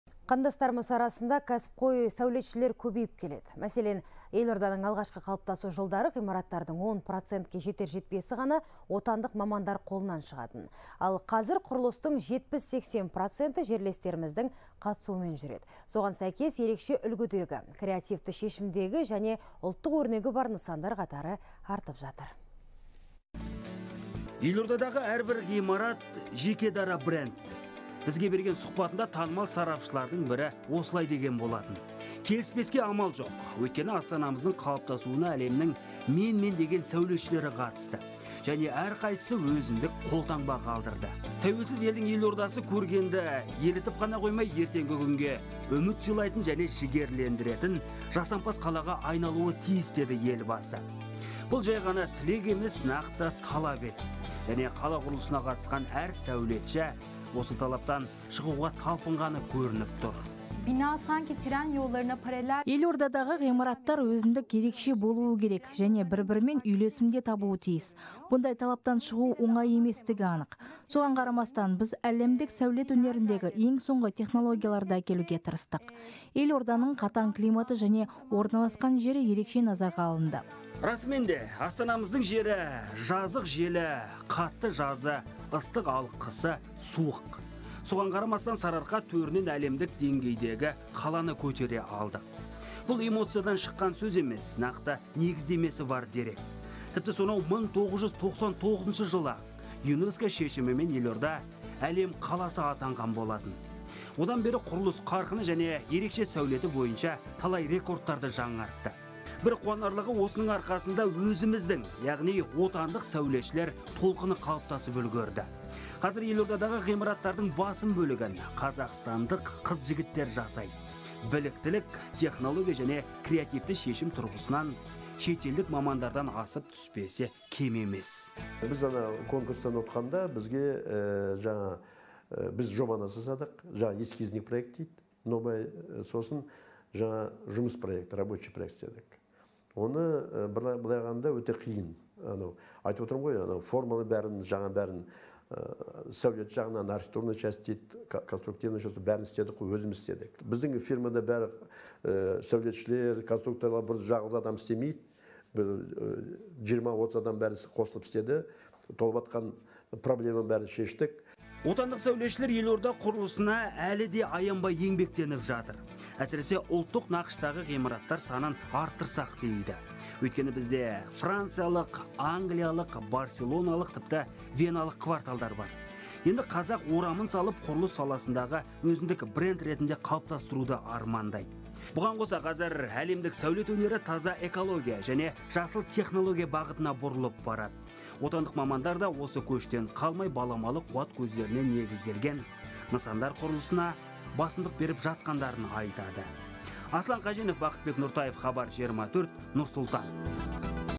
Мамандар «Әлем қаласы» атанған елорда сәулетінің ерекшеліктерін атады
Қандастарымыз арасында кәсіпқой сәулетшілер көбейіп келеді.

Мәселен, елорданың алғашқы қалыптасу жылдары ғимараттардың 10 процентке жетер-жетпесі ғана отандық мамандар қолынан шығатын. Ал қазір құрылыстың 70-80 проценті жерлестеріміздің қатысуымен жүреді. Соған сәйкес ерекше үлгідегі, креативті шешімдегі және ұлттық өрнегі бар нысандар қатары артып жатыр.